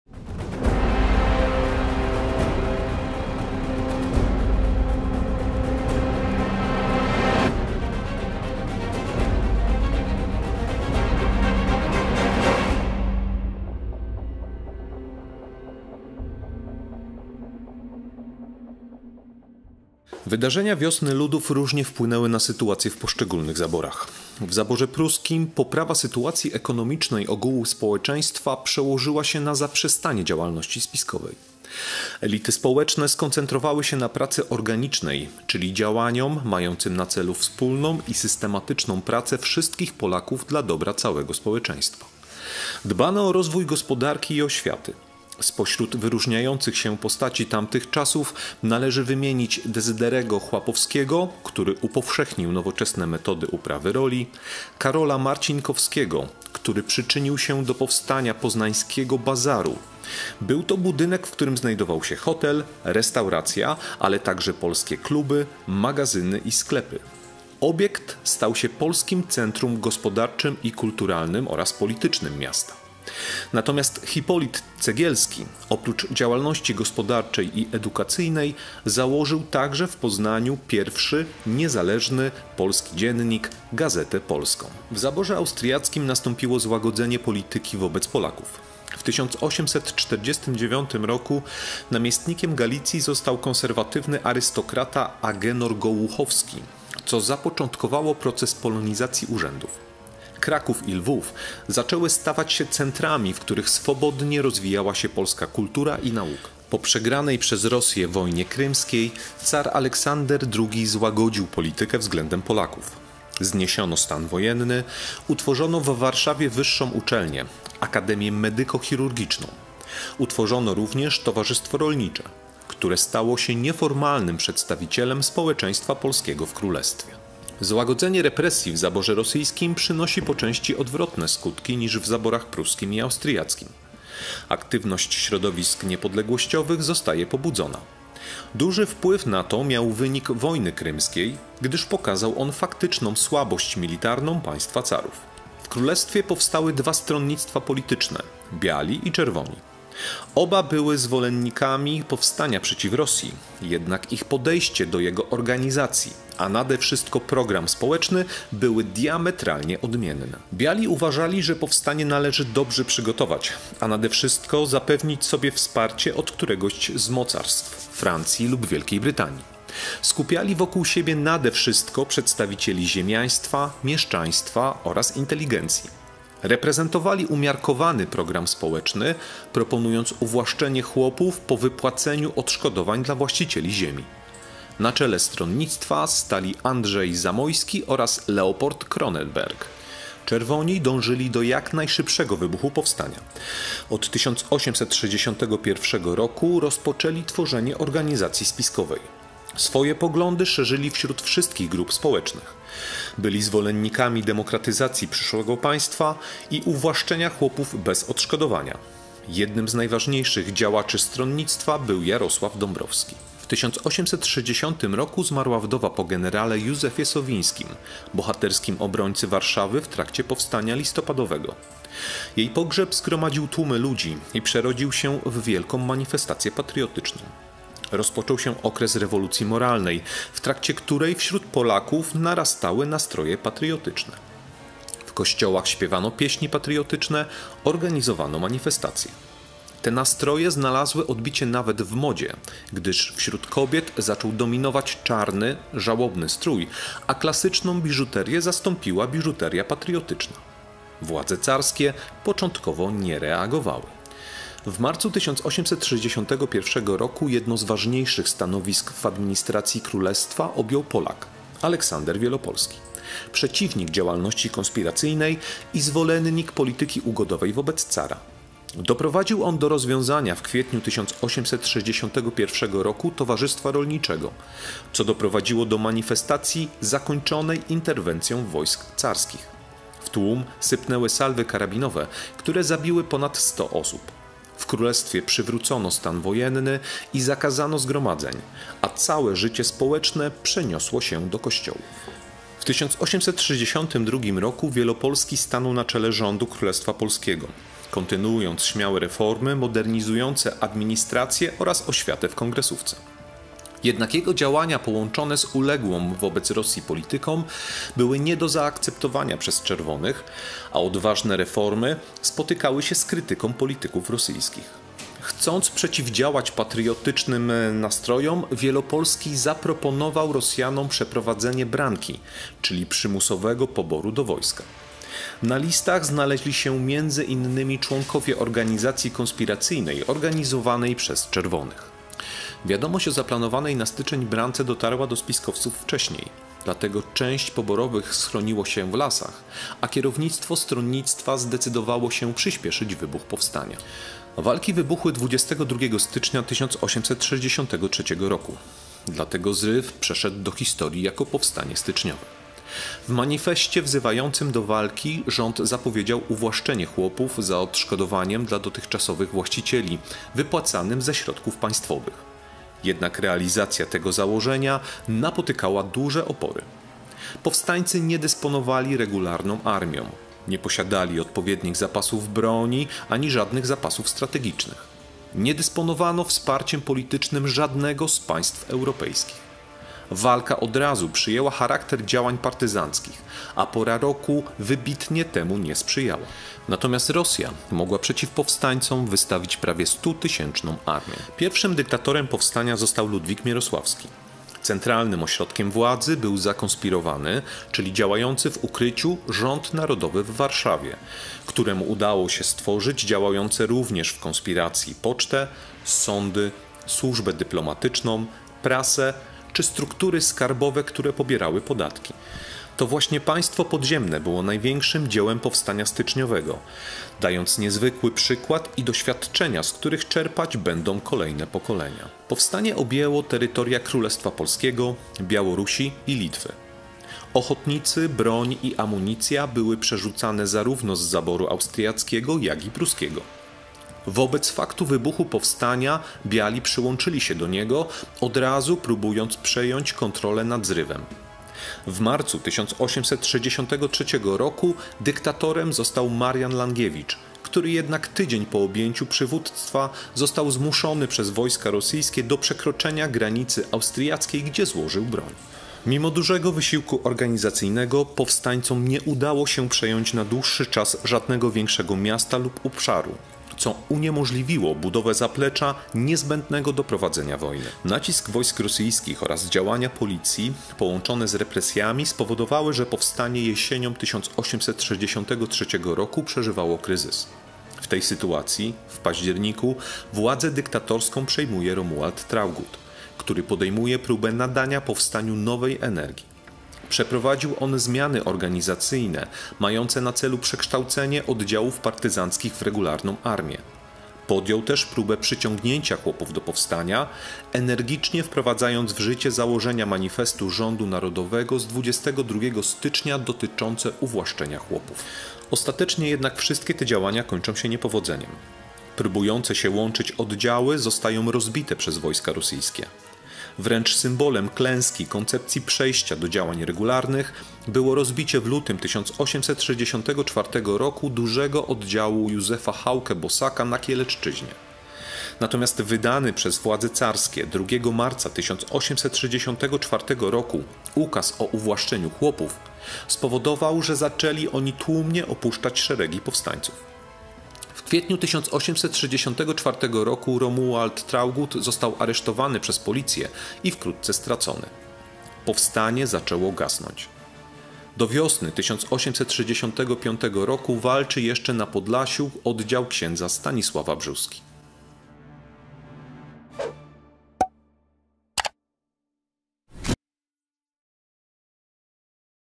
Powstanie styczniowe - Lekcje historii pod ostrym kątem - Klasa 7
Rozdział IV. Ziemie polskie po Wiośnie Ludów. Powstanie styczniowe. Wczoraj i dziś, wyd. Nowa Era

Lekcja historii obejmująca Powstanie styczniowe. W niej dowiecie w jaki sposób przebiegało powstanie styczniowe.  Poznacie jakie były czynniki determinujące wybuch powstania styczniowego. Usłyszycie w jakich okolicznościach wybuchło i przebiegało powstanie styczniowe. Zobaczycie jak rozstrzygnięta została sprawa chłopska.